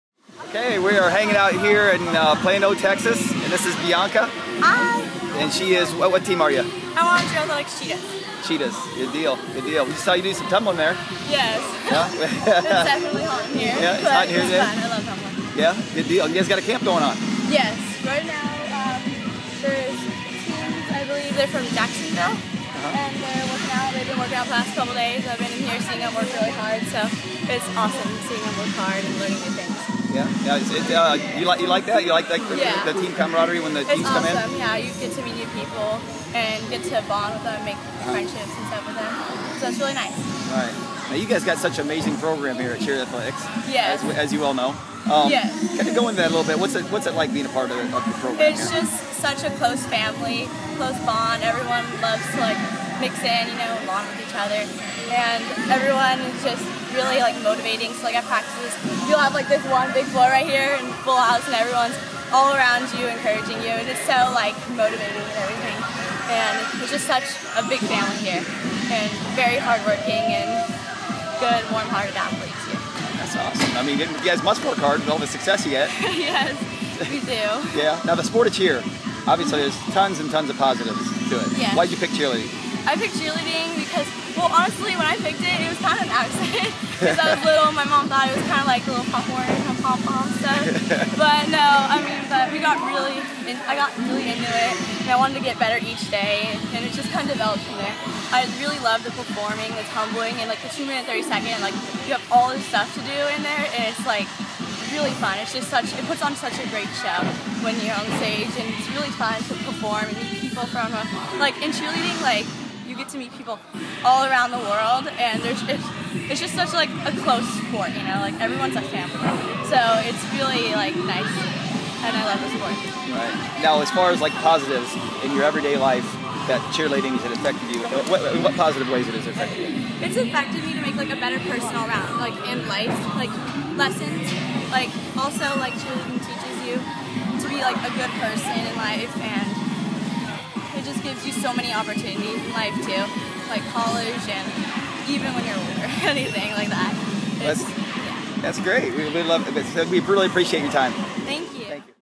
Bianca Treger
Bianca Treger is an outstanding athlete representing the Cheer Athletics Cheetahs